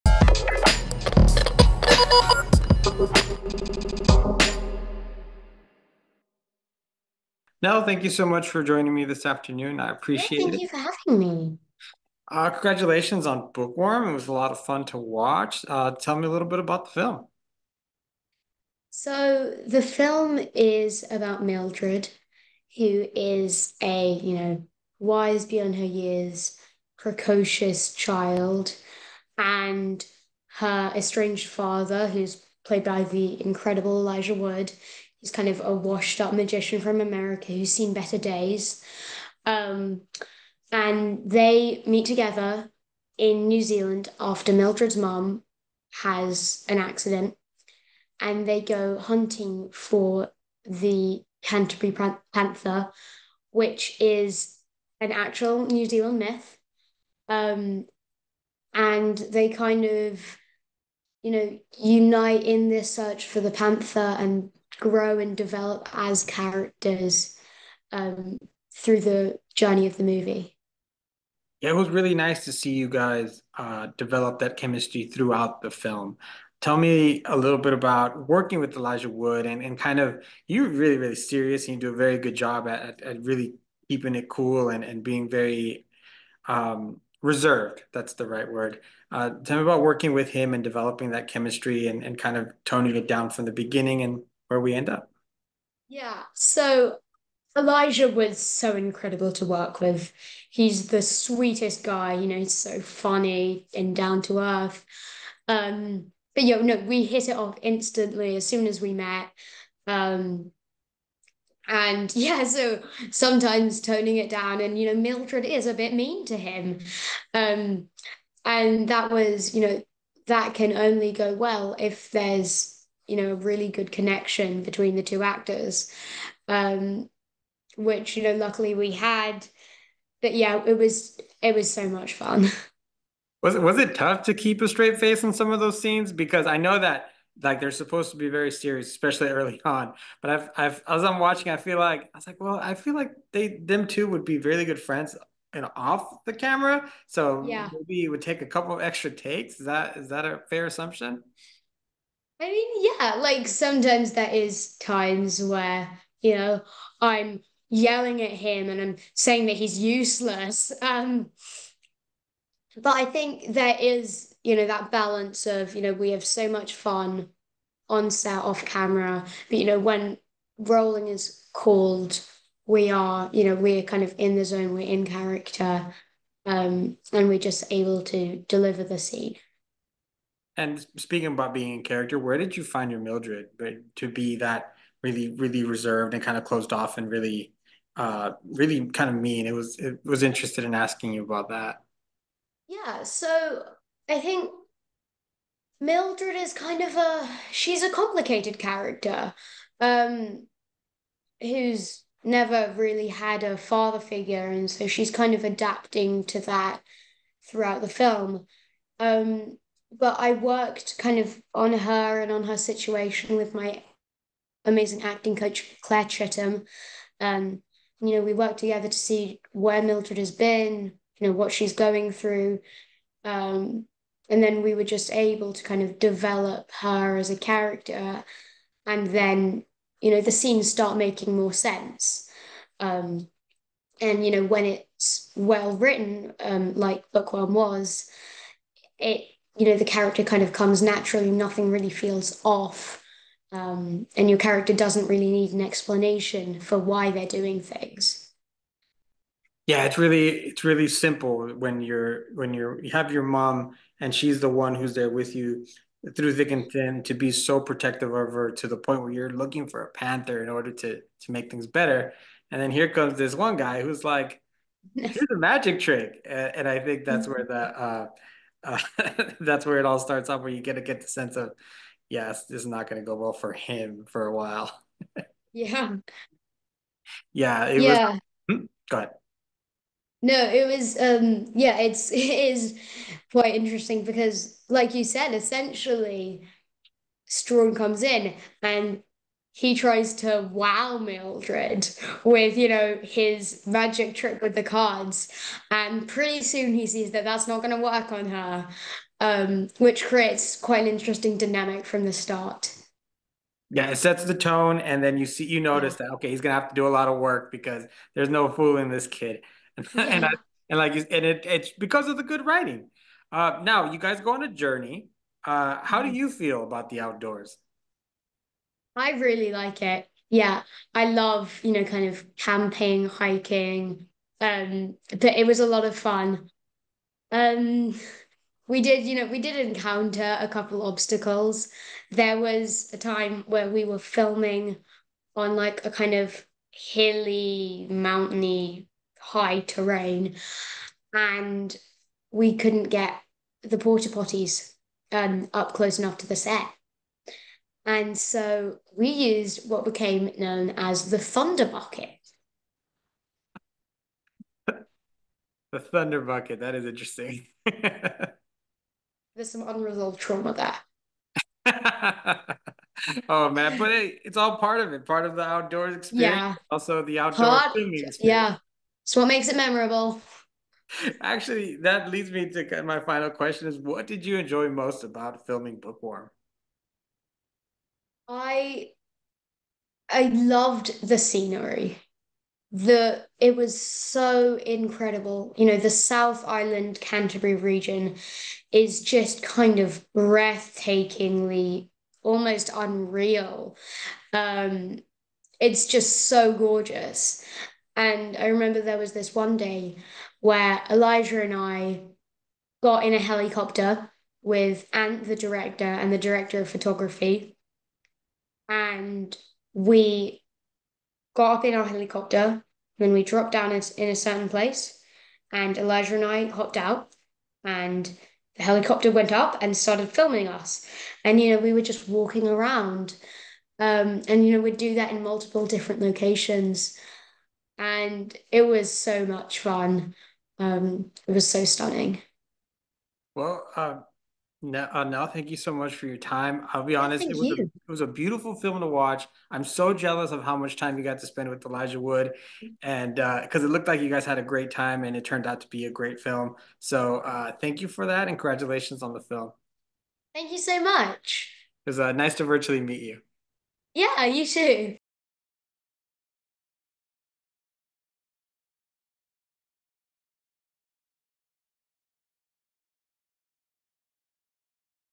Bookworm: Nell Fisher | THS Interview
Manny spoke with actress, Nell Fisher. She is one of the stars in the new Vertical feature comedy, action, and adventure film, Bookworm. The film is now in theaters and On Demand.

Bookworm follows 11-year-old Mildred (Nell Fisher) whose life takes a wild turn when her estranged father, Strawn Wise (Elijah Wood), a washed-up Las Vegas illusionist, suddenly reappears. Years ago, after a magical encounter with the once-charming Strawn, Mildred's mom moved back to New Zealand. Now, a comical mishap leaves Strawn caring for Mildred while her mom is hospitalized. Their first adventure? Camping to hunt for the mythological Canterbury Panther. As this mismatched duo navigates the wilderness, magic tricks, and an extraordinary quest, hilarity ensues, and they might just find a way to piece their family back together.